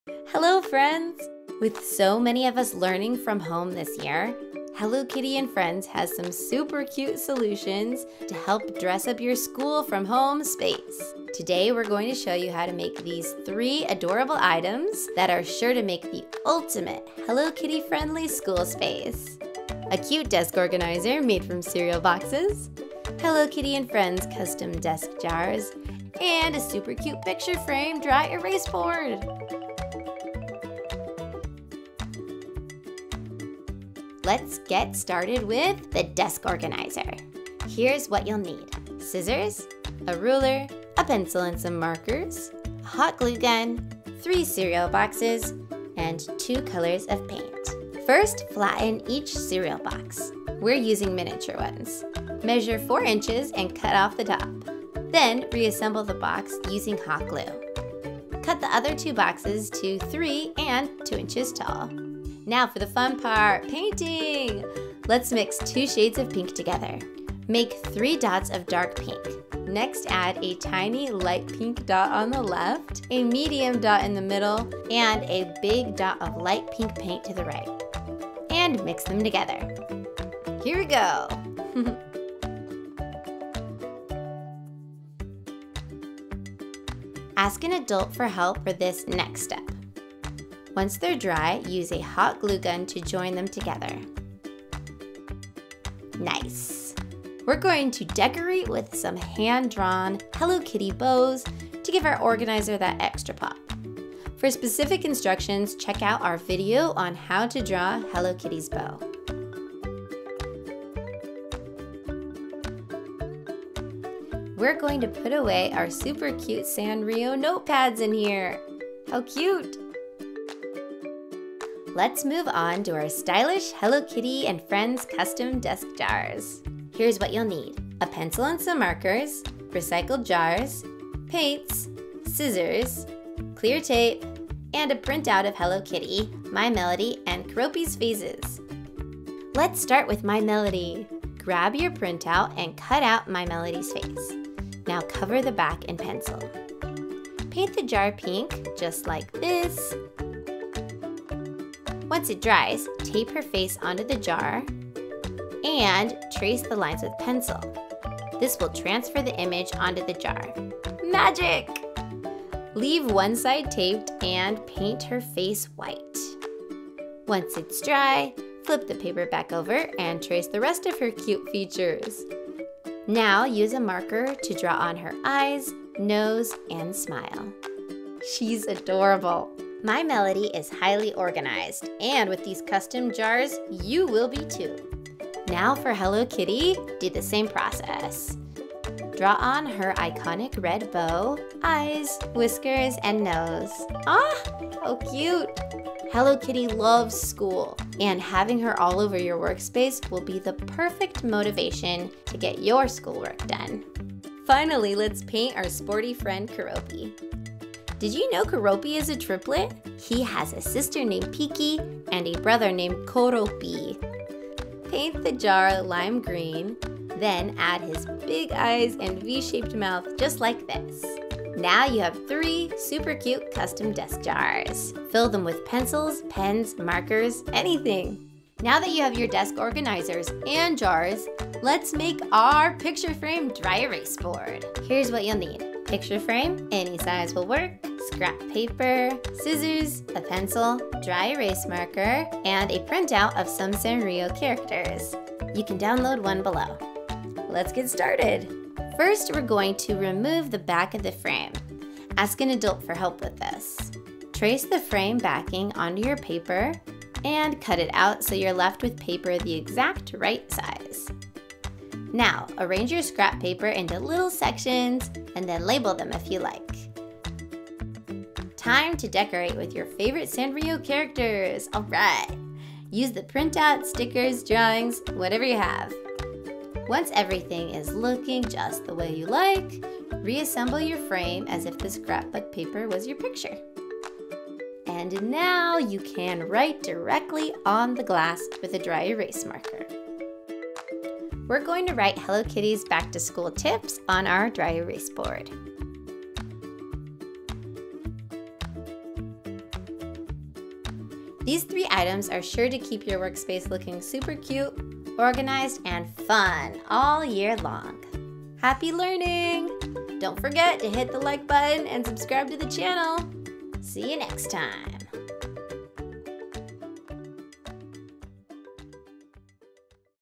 DIY Desk Decor Ideas to Make Your School Space Fun & Friendly | Hello Kitty Crafts
Learn how to make your school space fun and friendly with these Hello Kitty and Friends DIY desk decor ideas. With so many learning from home while back to school, Hello Kitty and Friends have some supercute desk decor DIY crafts to dress up your virtual classroom school space! We'll show you how to make these three adorable desk decorations that are sure to make the ultimate Hello Kitty-friendly school desk setup. A cute desk organizer made from cereal boxes, Hello Kitty and Friends custom desk jars and a supercute picture frame dry erase board.

Let’s start with the desk organizer!

Materials Needed:
-Scissors
-Ruler
-Pencil
-Markers
-Hot glue gun
-Three cereal boxes
-2 colors of paint

Step-by-Step Instructions:
(00:00) Start
(00:50) Step 1: First flatten each cereal box. We’re using miniature ones!
(00:53) Step 2: Measure four inches and cut off the top
(00:56) Step 3: Reassemble the box using hot glue
(00:59) Step 4: Cut the other two boxes to 2 and 3 inches tall

Now for the fun part - painting!!
(01:05) Step 5: Mix two shades of pink together. Make three dots of dark pink
(01:11) Step 6: Next add a tiny light pink dot on the left, a medium dot in the middle and a big dot of light pink paint on the right. Now mix them together!
(01:20) Step 7: Paint all three boxes
Ask an adult for help for this next step!
(01:29) Step 8: Once your boxes are dry, use a hot glue gun to join them together.
(01:38) Step 9: We’re going to decorate with some hand-drawn Hello Kitty bows to give our organizer that extra pop.

Let’s move on to our stylish Hello Kitty and Friends custom desk jars.

Materials Needed:
-Pencil
-Markers
-3 Recycled jars
-Paints
-Scissors
-Clear tape
-Printout of Hello Kitty, My Melody and Keroppi’s faces

Let’s start with My Melody!
Step-by-Step Instructions:
(02:21) Step 1: Grab your print out and cut out My Melody’s face
(02:25) Step 2: Now cover the back in pencil
02:28) Step 3: Paint the jar pink
(02:32) Step 4: Once it dries, tape her face onto the jar and trace the lines with a pencil. This will transfer the image onto the jar
(02:43) Step 5: Leave one side taped and paint her face white
(02:48) Step 6: Once it’s dry, flip the paper back over and trace the rest of her cute features
(02:54) Step 7: Now use a marker to draw on her eyes, nose and smile

Hello Kitty
Step-by-Step Instructions:
(03:07): Do the same process as above and draw on her iconic red bow, eyes, nose and whiskers

Finally, let’s paint our sporty friends, Keroppi!
Step-by-Step Instructions:
 03:36): Paint the jar lime green and then add his big eyes and v-shaped mouth

Check out these supercute Hello Kitty and Friends pens to add to your stylish mason jars!

Picture Frame Dry Erase Board
Materials Needed:
-Picture frame (any size will work)
-Scrap paper
-Scissors
-Pencil
-Dry Erase board marker
-Print out of Sanrio characters

Download this PDF of the Sanrio characters to decorate your frame

Step-by-Step Instructions:
(04:09) Step 1: First we’re going to remove the back of the frame
Ask an adult for help with this next step.
(04:14) Step 2: Trace the frame onto your paper and cut it out so you’re left with paper the exact right size
(04:23) Step 3: Arrange your scrap paper into little sections and then label them if you’d like.
(04:33) Step 4: Use the printouts, stickers, drawings or whatever you have available
(04:37) Step 5: Once everything looks just the way you like, reassemble your frame as if the paper was your picture!
-04:46) Step 6: Now you can write directly on the glass with a dry erase marker

These three items are sure to make your virtual learning space look supercute, organized and fun!

SANRIO and HELLO KITTY are trademarks of Sanrio Company Ltd. Character images are protected by Copyright.